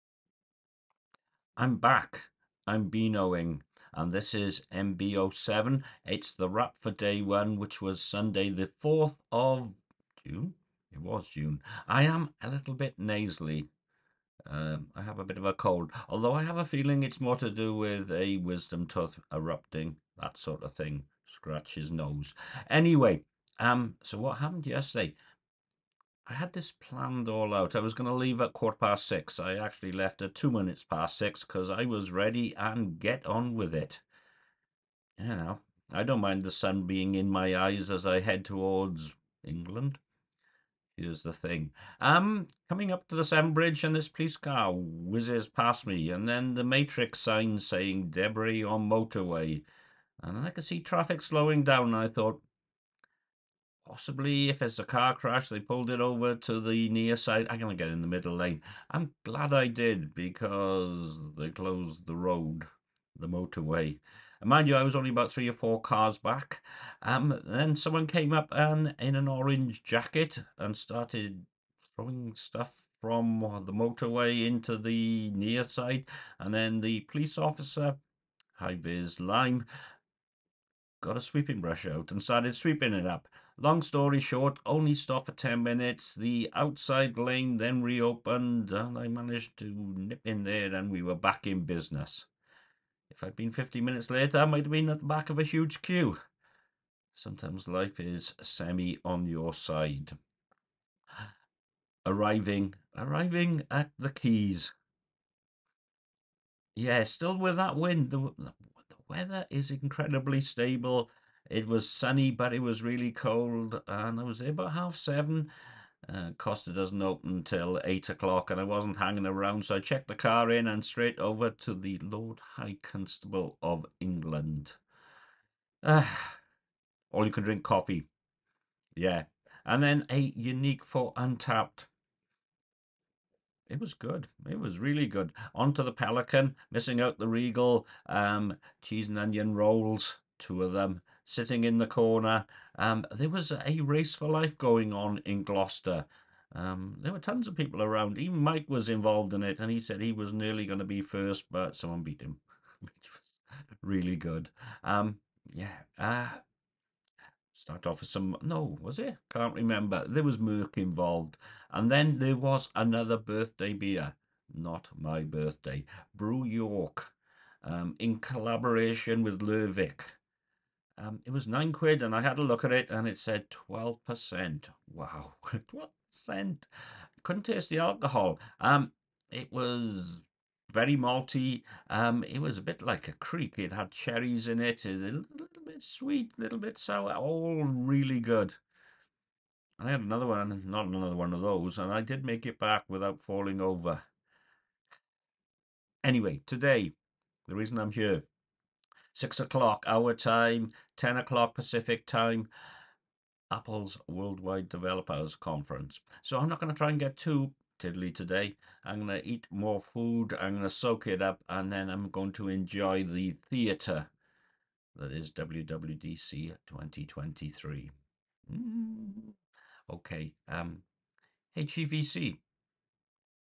20230604 mb07 Day 01 Wrap
When you leave 15 minutes early and you only get delayed for 10 minutes on the bridge. Also being in the middle lane helped too. Still with the cold wind again. The Lord High Constable and The Pelican while there was a race for life. Cheese and Onion Rolls with a Pint of Murk. Then there was another Birthday Ale, this time from Brew York. A Collab with Lervig at 12% with Cherries.